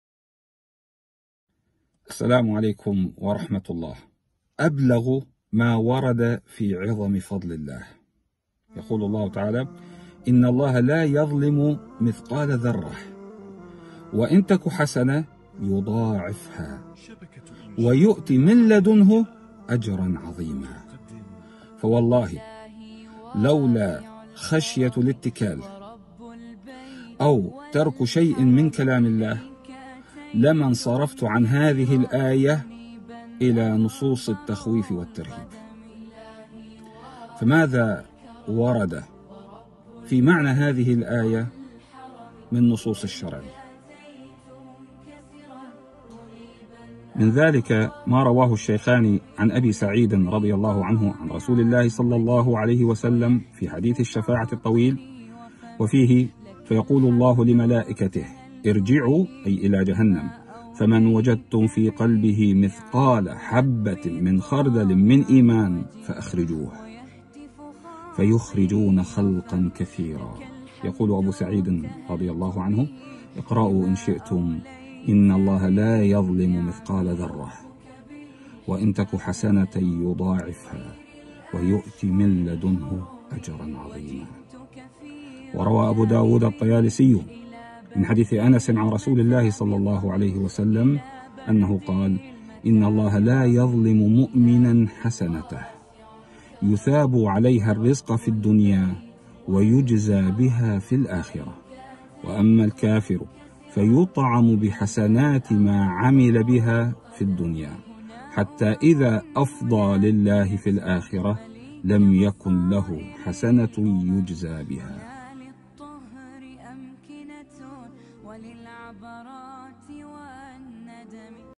عظم فضل الله - عبدالناصر أبومصطفى
عظيمُ فضلِ اللهِ تعالى، مِن خلالِ قولِهِ تعالى: {إِنَّ ٱللَّهَ لَا یَظۡلِمُ مِثۡقَالَ ذَرَّةࣲ، وَإِنْ تَكُ حَسَنَةࣰ یُضَـٰعِفۡهَا، وَیُؤۡتِ مِن لَّدُنۡهُ أَجۡرًا عَظِیمࣰا} (النِّساء:40)
مدة المقطع: دقيقتان